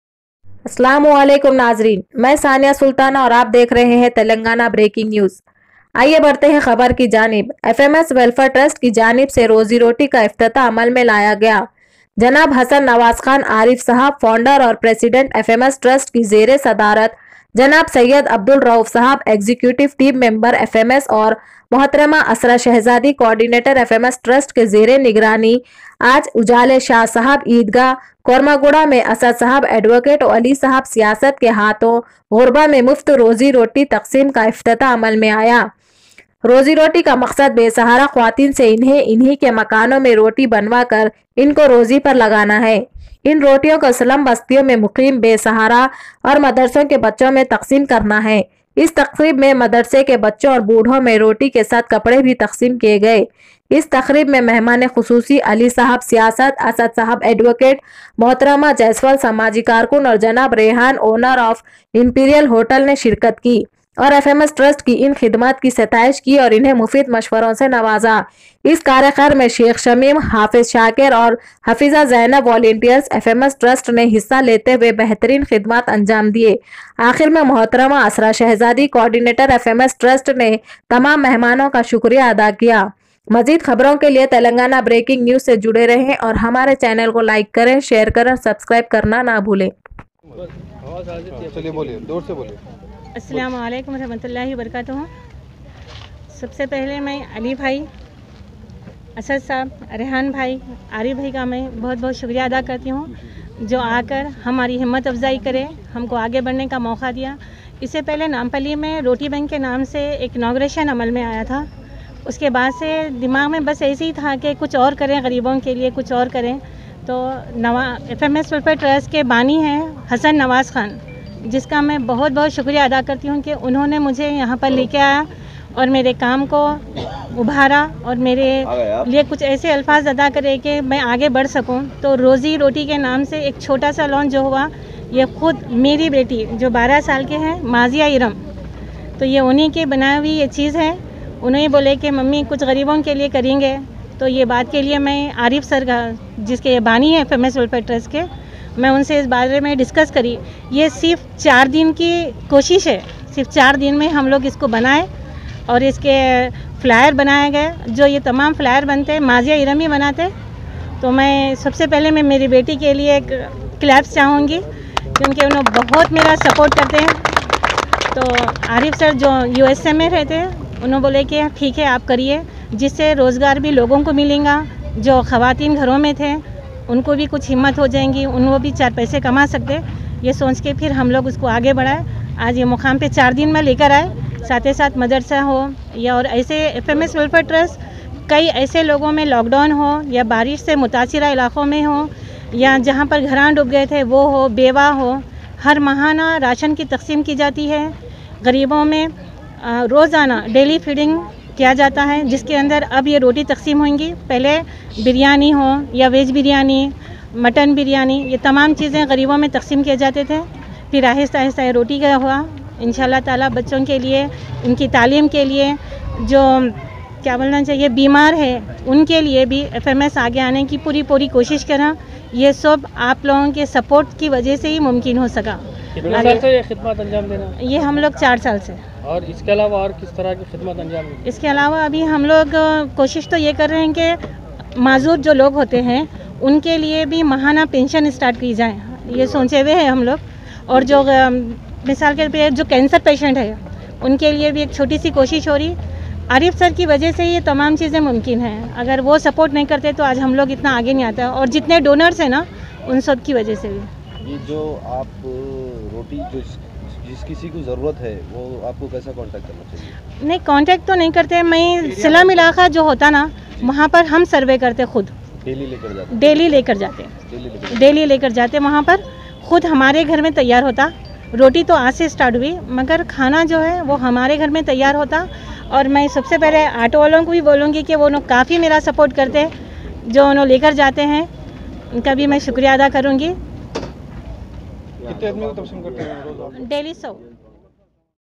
FMS Welfare Trust ki Janib Se ROZI ROTI ka Iftetah Amal Mein Aya GHARIB AUR BESAHARA Logao Ke Liye
Fms welfare Trust ki janib se Rozi Roti ka Iftetah Amal Mein Aya.
Janab Hassan Nawaz Khan (Arif Sahab) Founder and president fms trust ki zerey sadarat Janab syed Abdul Rouf Sahab executive Team Member fms or mohatrama Asra Shahzadi Coordinator Fms Trust ke Zeray nigrani Aaj Ujale Shah Sahab Eidgah Kormaguda me Asad Sahab Advocate-o-Ali Sahab Siasat Ke Hatau Ghurba me Muft Rozi Roti taqseem ka ifteta Amal me Aaya.Rozi roti ka maqsad be sahara  khwateen se inhi ke makanau me roti banwa kar inko rozi par lagana hai in rotiyau ko slum bastiyau me muqeem besahara or madarsau ke bachau me taqseem karna hai.is taqreeb me madarse ka bachau or badau me roti ke sath sath kapdre bhi taqseem kiye gaye.is taqreeb me mehmane qususi Ali sahab siasat,Asad Sahab Advocate, mohatrama Jasval Samaji karkun or janab Rehan owner imperial Hotel ne shirkat ki or fms trust ki in khidmaat ki sataish ki or inhe mufeed mashwerau se nawaza..is kaare me mazia erum, Shaik shameem, hafiz shaker or hafiza Zainab volunteers fms trust ne hissa lete huwe behtreen khudmat anjam diye..akhir me mohartama Asra shahzadi coordinator fms trust ne tamam mehmanau ka shukiria ada kiya. |@TELANGANA_BREAKING_NEWS.| 14/01/2021. TNB NEWS.